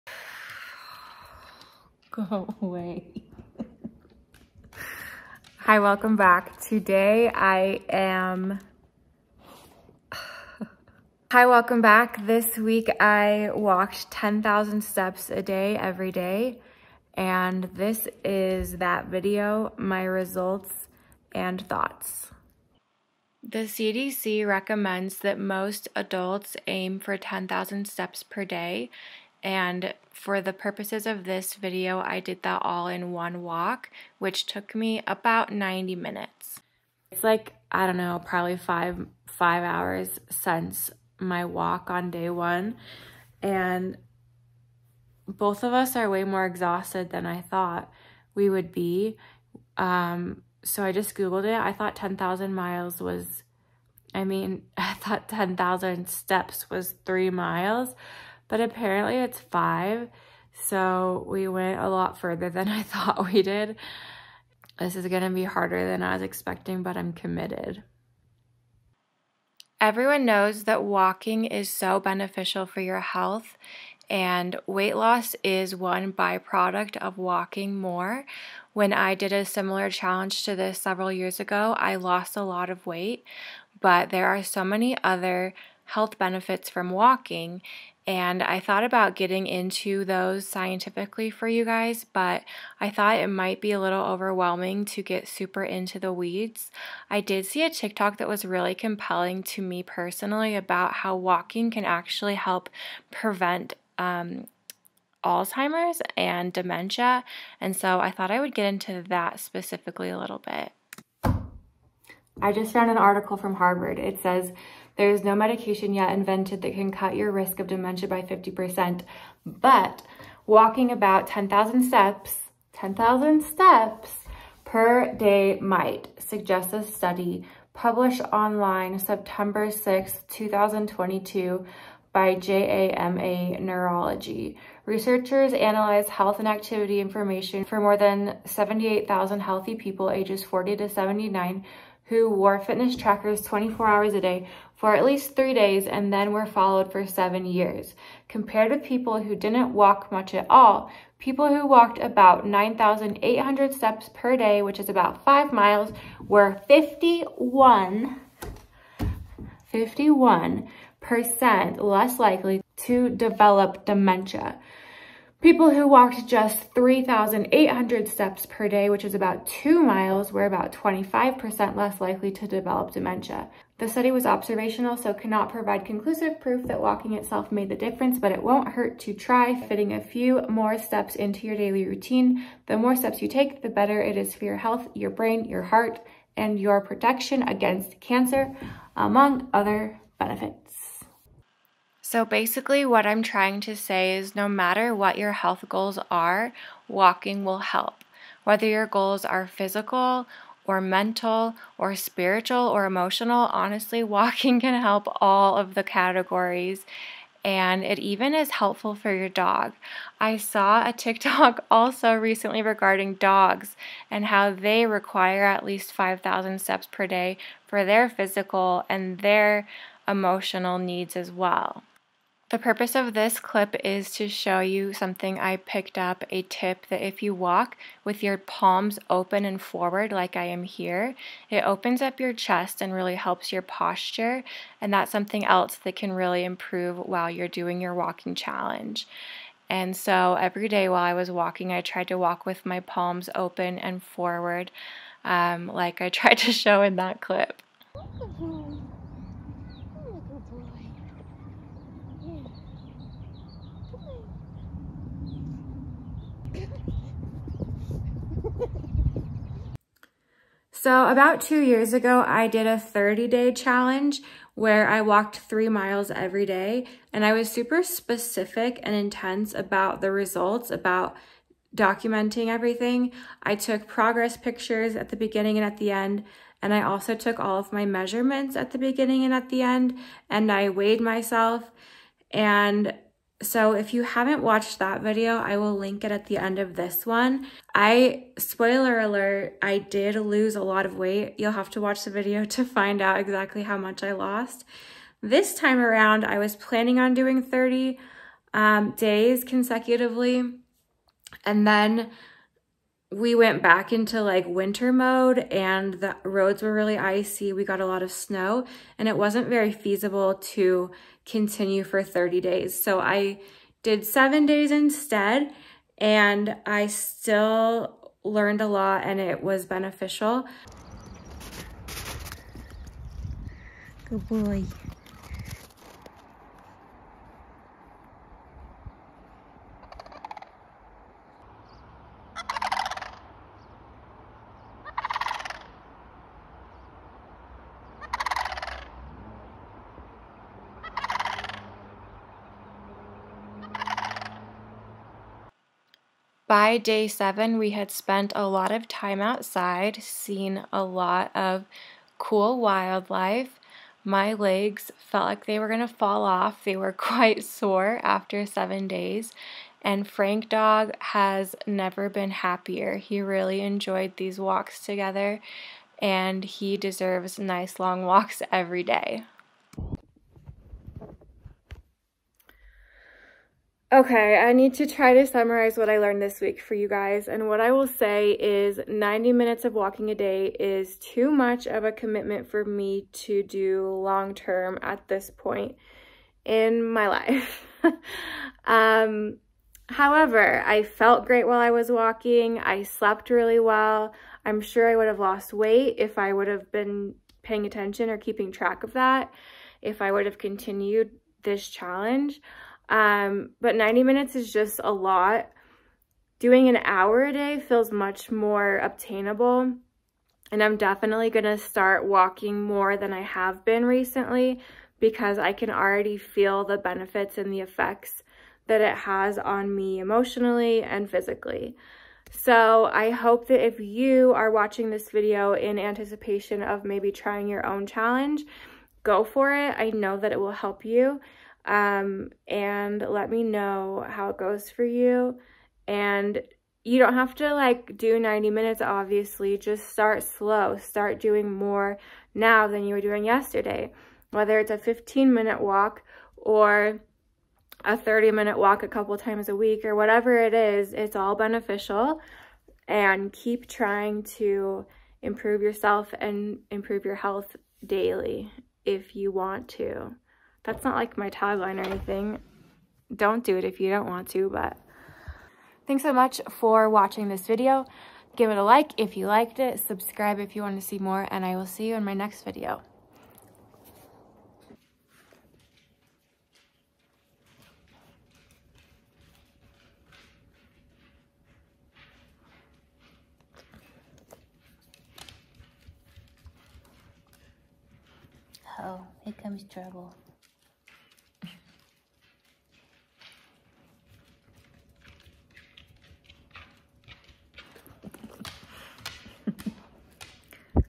Walking 10,000 Steps (5 Miles) a Day for a Week
Walking 10,000 steps a day which is 5 miles challenge!